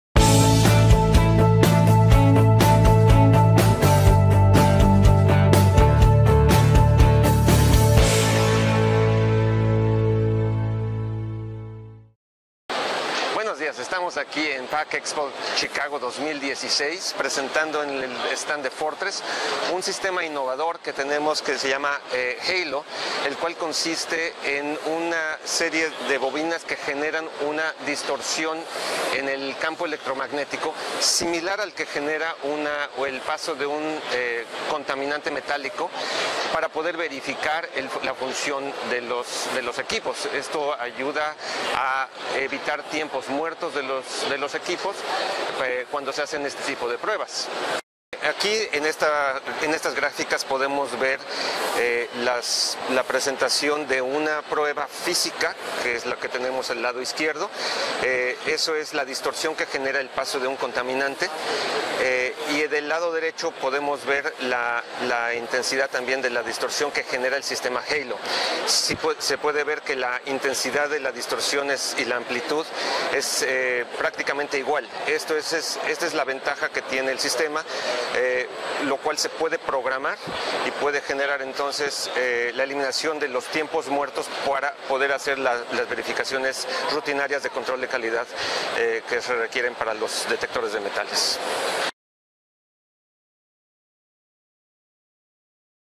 Halo de Fortress en Pack Expo 2016
La revolucionaria innovación con patente en trámite HALO  es un sistema de prueba de funcionamiento automático del equipo de detección de metales. Está diseñado para generar automáticamente las señales emitadas  por el paso de materiales ferrosos , no ferrosos y acero inoxidable a través de nuestros detectores de metales Fortress.